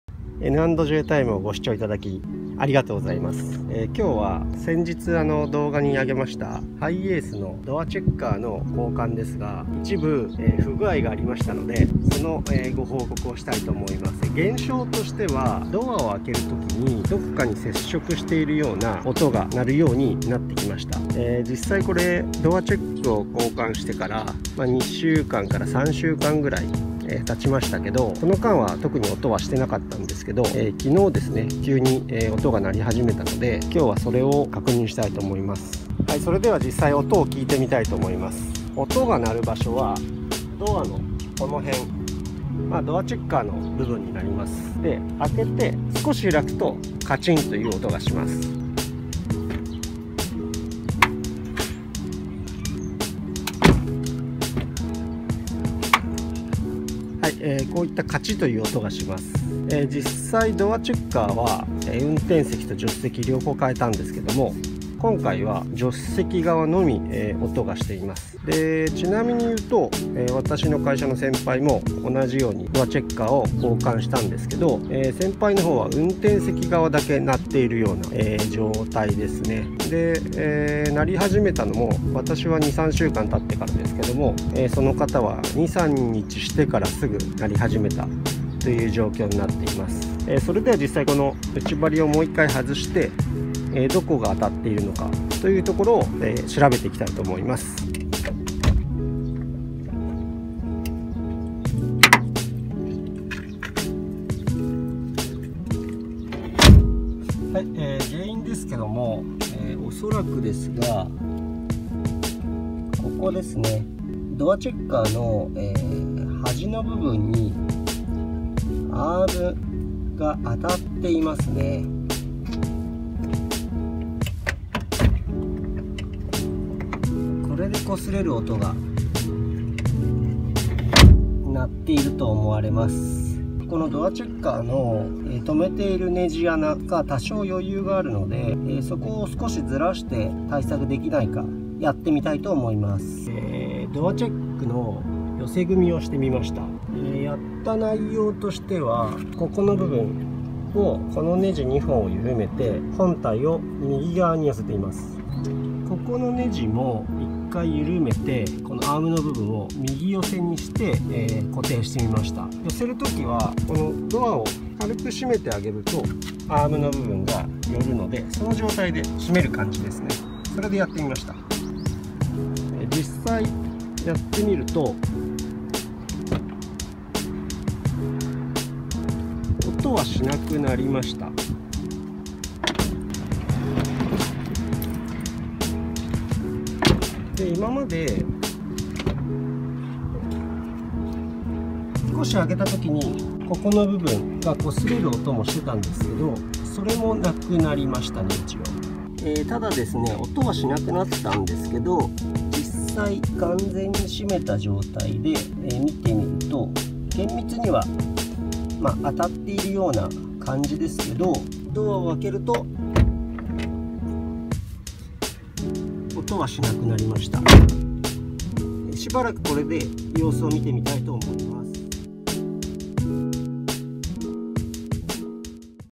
ハイエースドアチェッカー 不具合報告
いつも N&J TIME をご視聴頂きありがとうございます。
本日の動画は前日ご紹介しましたハイエースのドアチェッカー交換による不具合が見つかりましたのでご報告いたします。
安全面などの影響はありませんが、異音がするため暫定的に対応しました。
また、他に不具合、対策をご存じの方がいらっしゃいましたらコメント頂ければ幸いです。